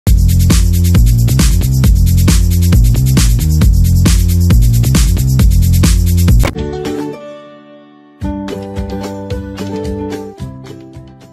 Large Capacity Japanese Style Oil Filter Bottle with Lid
This oil strainer pot helps us separate food from fat and reduce fat intake. Food grade premium stainless steel, high temperature resistant, corrosion-resistant, healthy material.